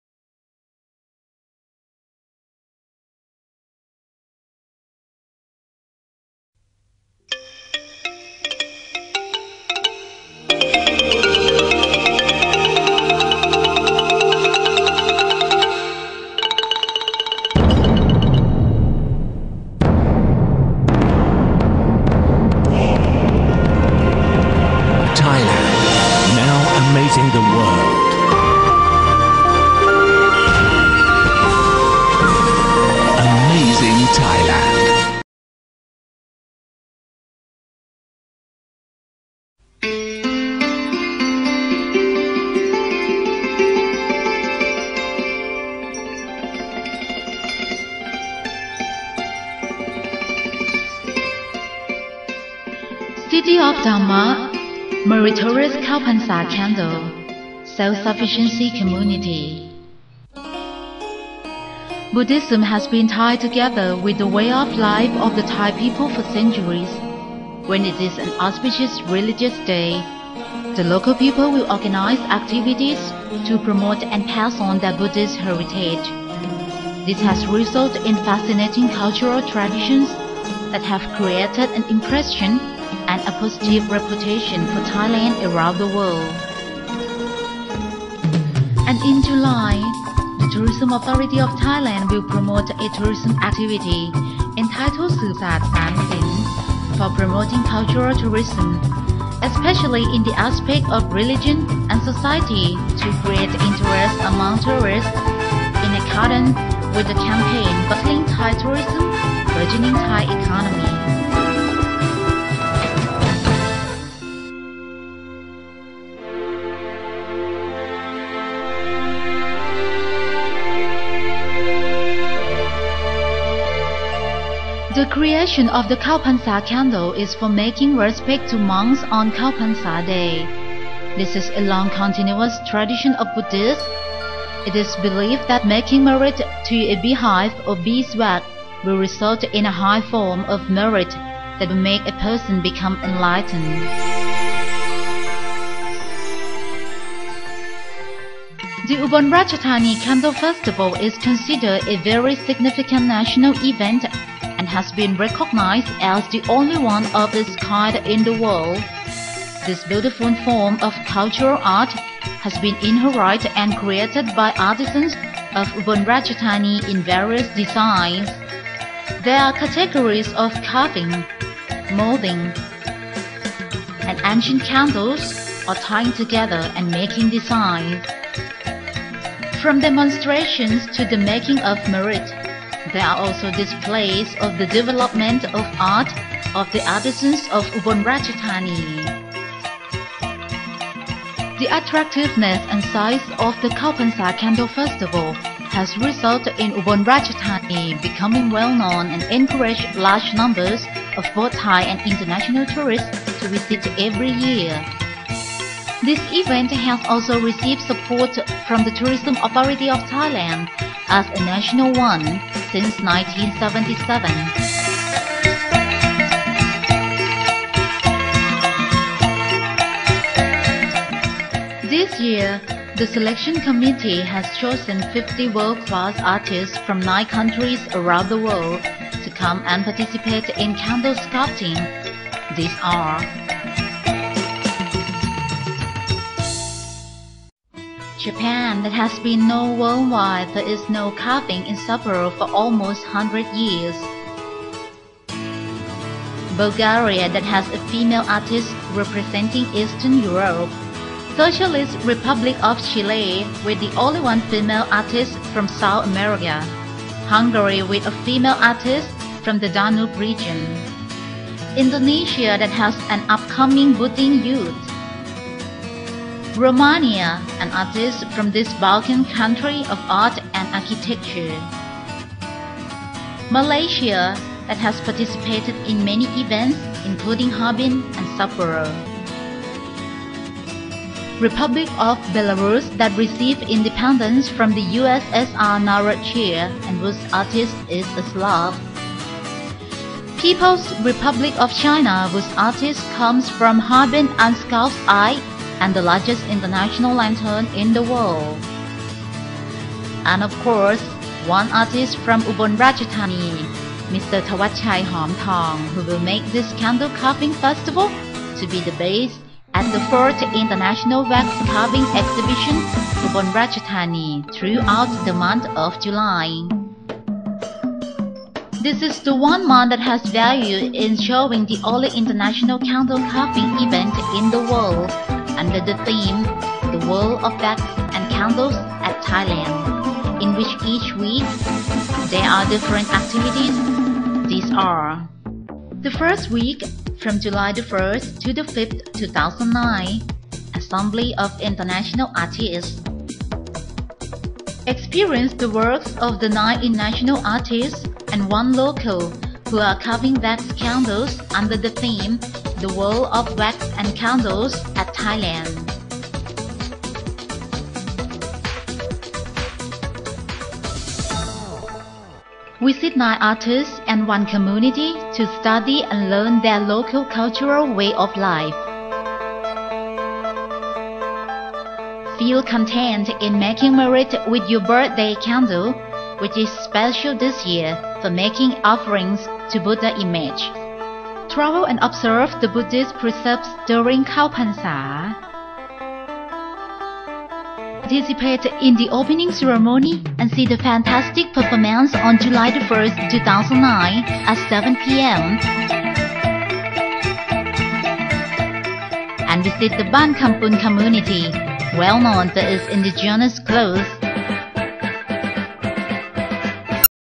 Thailand International Wax 2009 English Ver.
The Candle Wax Procession is one of the famous Buddhist ceremonies, performed nationwide at the beginning of the Buddhist Lent, fall into July every year. This wonderful festival is not only Buddhist ceremony but also tourism activity that is able to promote domestic tourism. One of the outstanding events is held in Ubon Ratchathani that has received great enthusiasm for years from both Thais and Overseas to experience the wonderful creation and masterpiece that are created by local artists, people in various community as well as international artists who come to participate in this event.